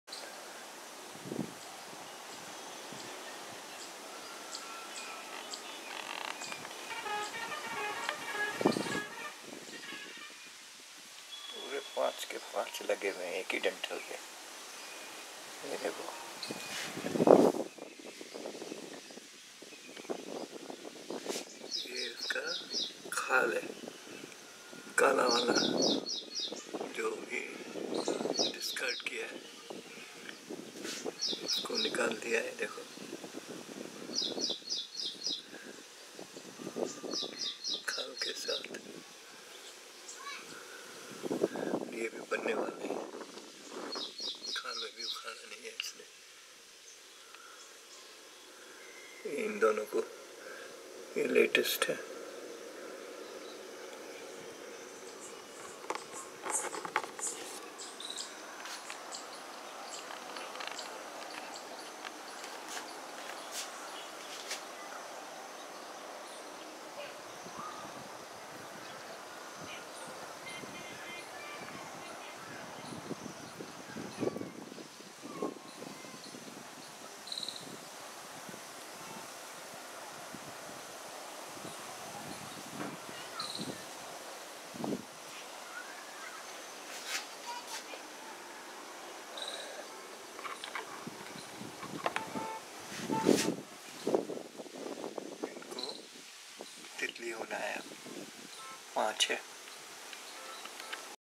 Butterfly pupae in captivity lined up on a single twig
About five years back I had set up a lab in my kitchen veranda for recording the life cycle and behaviours of butterflies and moths visiting my garden.  Many of these were uploaded. However, the emergence of an adult butterfly from pupa was by an error of switching to recording mode was missed. This year I find fewer butterflies in the garden. But was fortunate to  find some larvae on the only King-lemon tree probably because there are few birds visiting now.   Here is a video  with three fully formed pupae and two in the maturing state in  a single twig placed in the box.